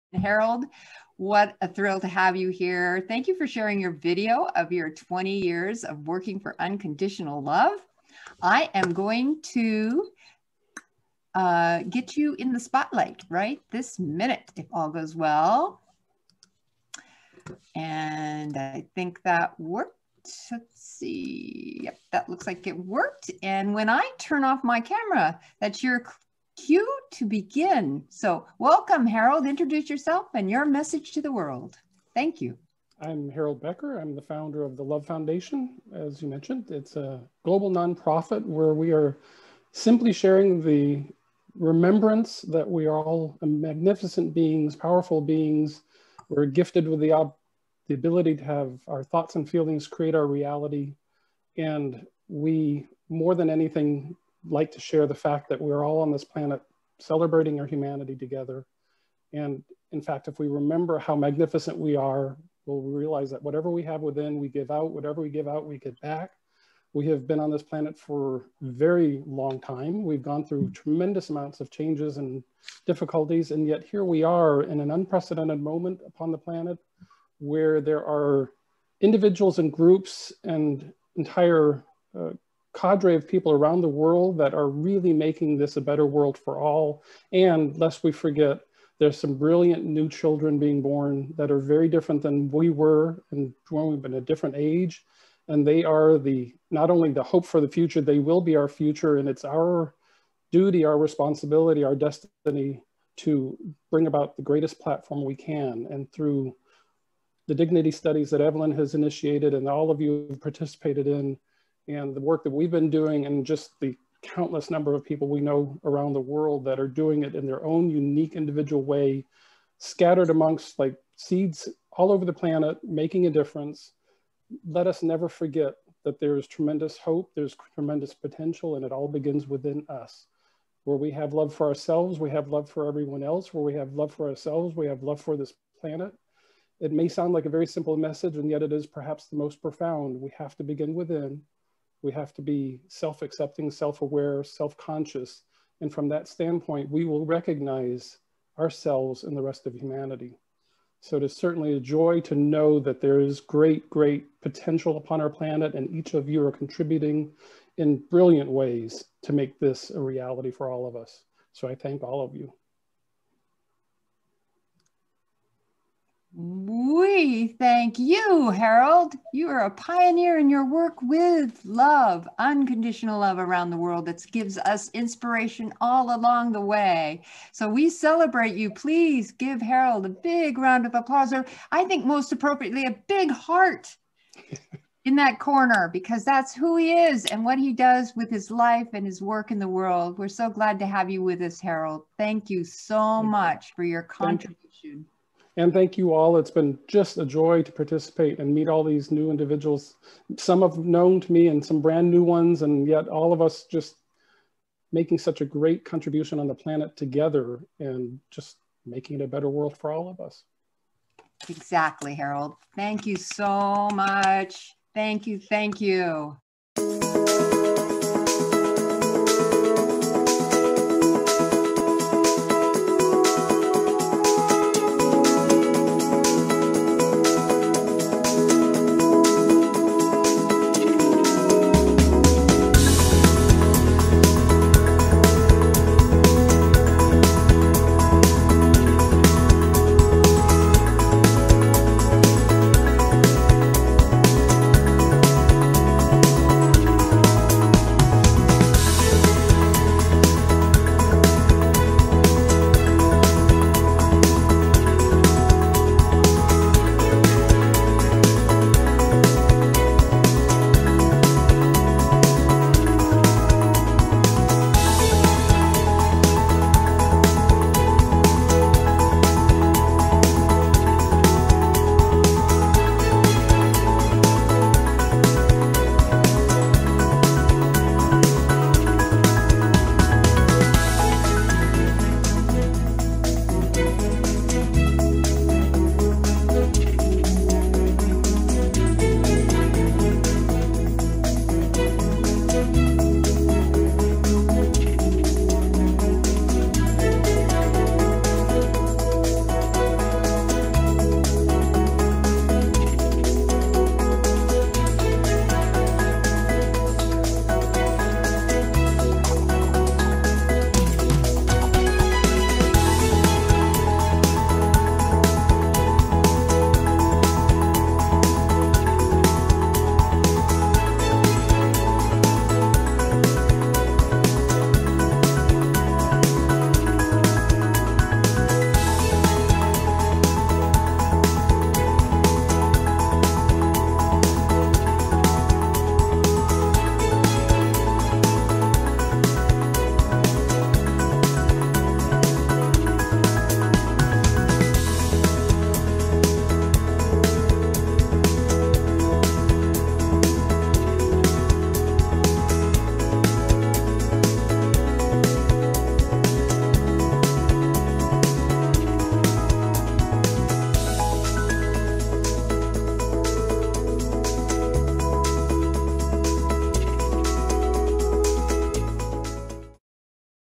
2020 Dignity Workshop 314: Message to the World from Harold Becker
This is Harold Becker's "Message to the World" on Day Three of the 17th Workshop on Transforming Humiliation and Violent Conflict at Columbia University in New York City, December 10 – 12, 2020. Due to the coronavirus pandemic, this conference had to take place online.

"Messages to the World" are a contribution to the World Dignity University Library of Ideas to be shared with the world and, hopefully, inspirational for future generations.

Harold Becker founded The Love Foundation, together with John T. Goltz, a globally recognized non-religious and non-political non-profit organization with the mission to "inspire people to love unconditionally." See "The Love Foundation — Celebrating 20 Years"